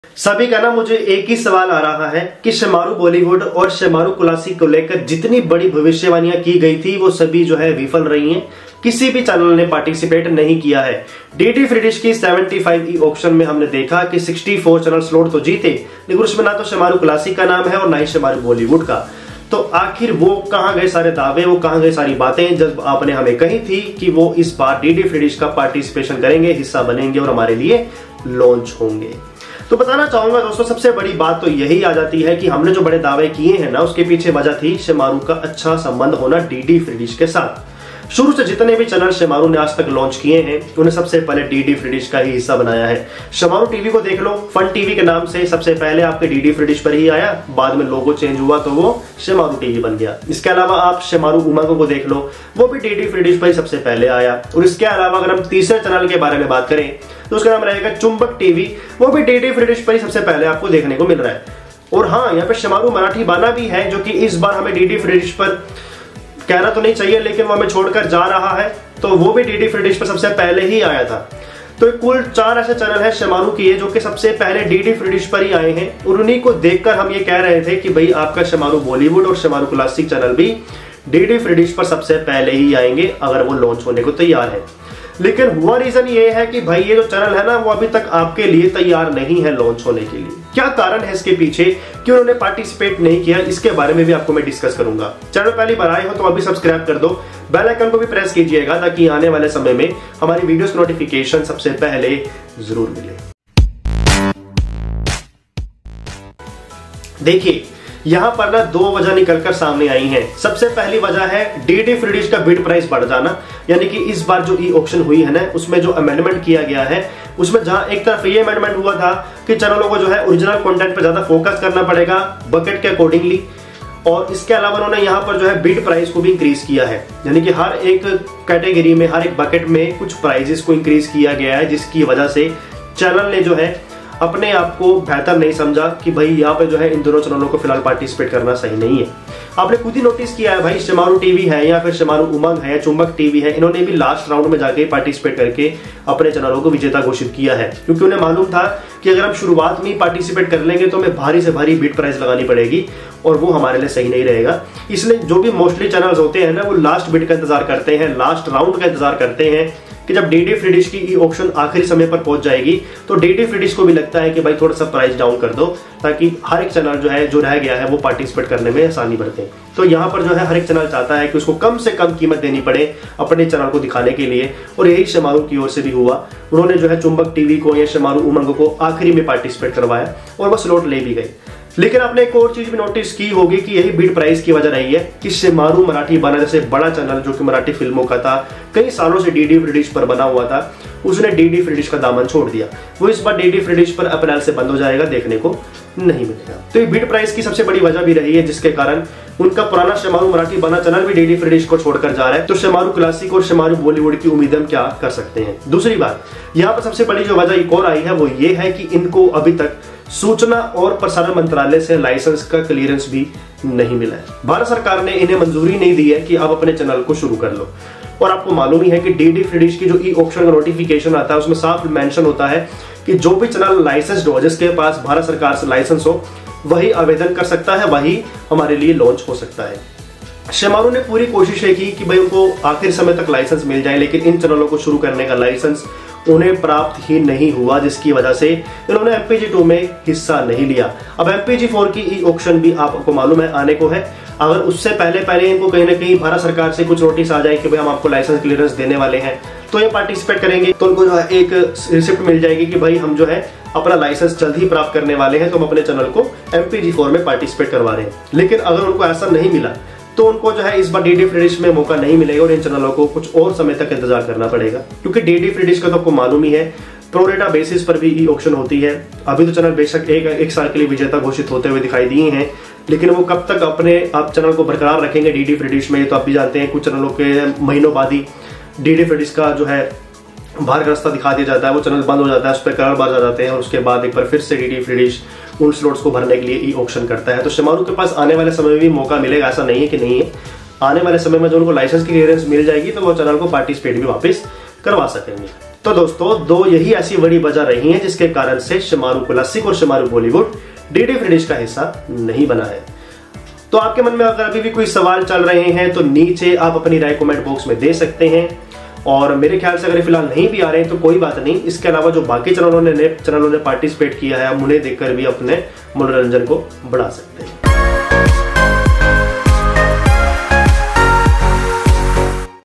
Why Shemaroo Bollywood & Shemaroo Classic not launching on DD Free Dish 😰
Namaskar Dosto,

Aaj hum aapko batayenge ki aakhir aisi kya wajah bani ki shemaroo bollywood or shemaroo classic ne participate nahi kiya. iski puri jankari ke liye video ko pura jarur dekhe.

Dhanyawad.